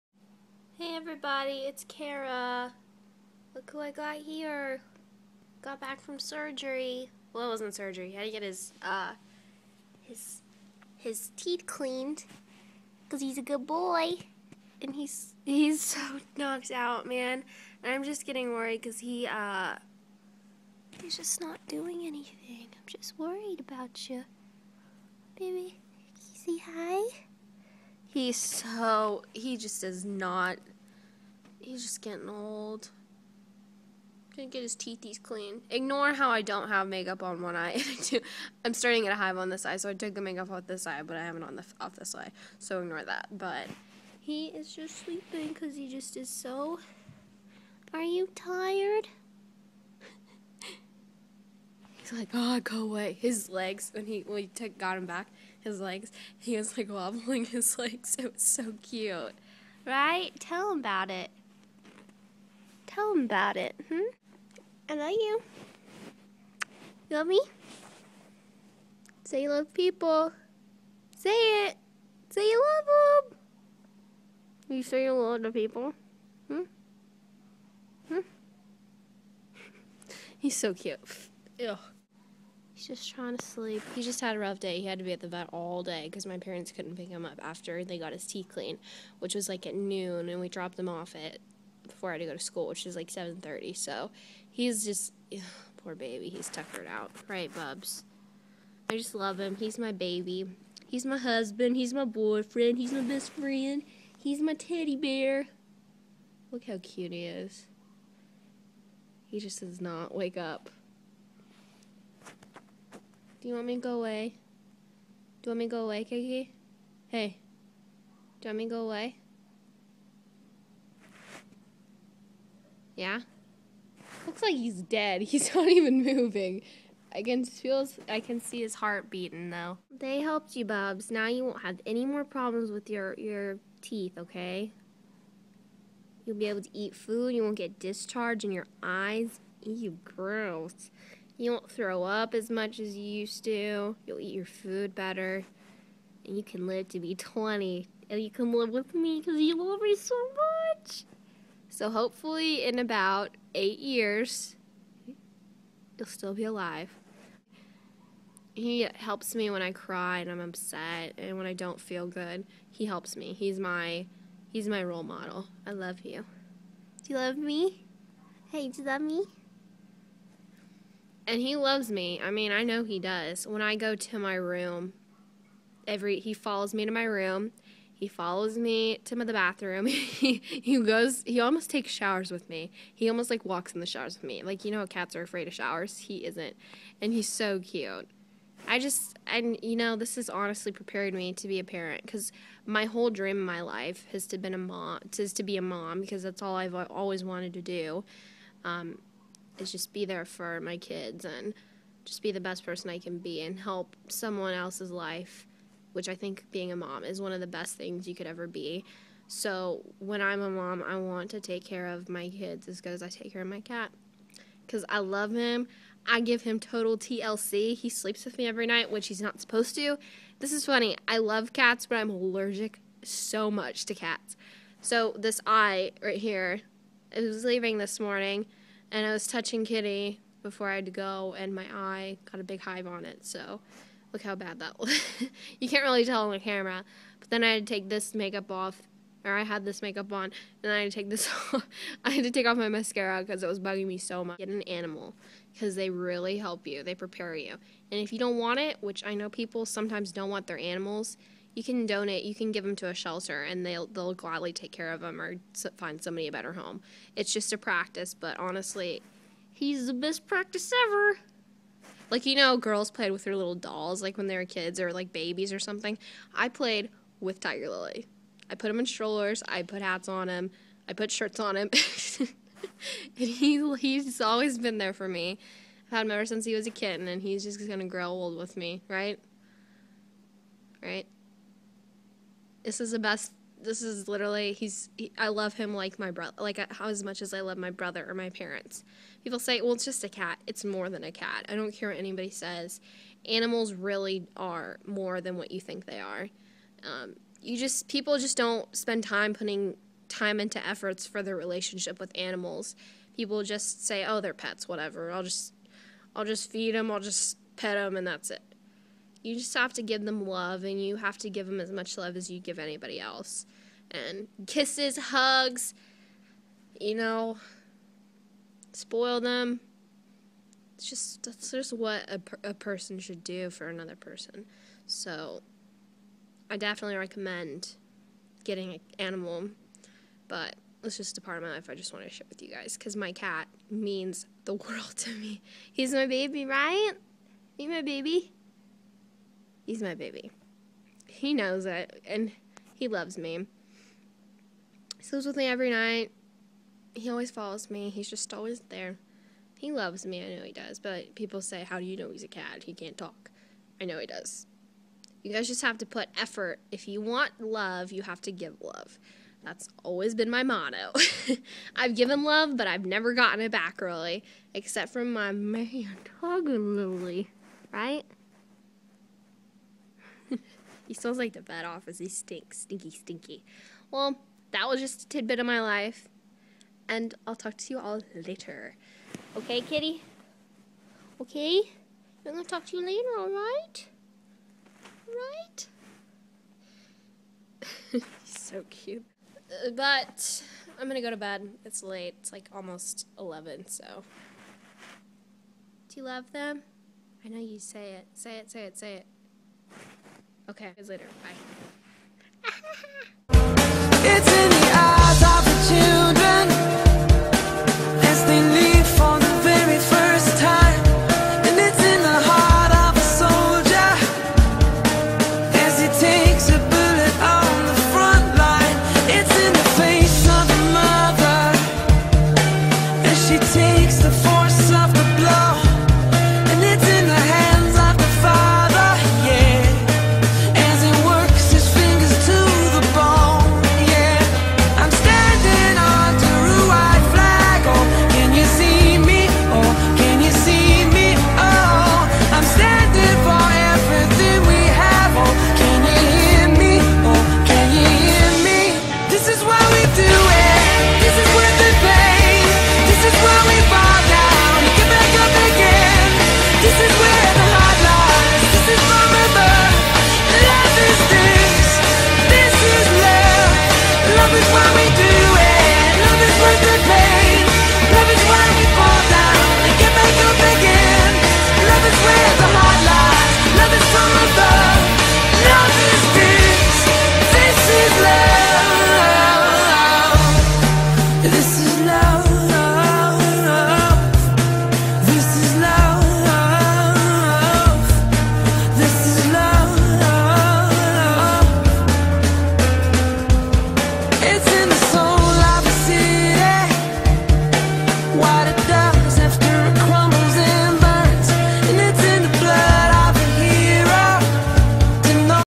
This is Love.
READ THIS!
I hope this video opens up your eyes to try and make a very good relationship with your pets. They are more than just pets. People forget that they have feelings and thoughts like people too, they can't be ignored. Love life and your animals.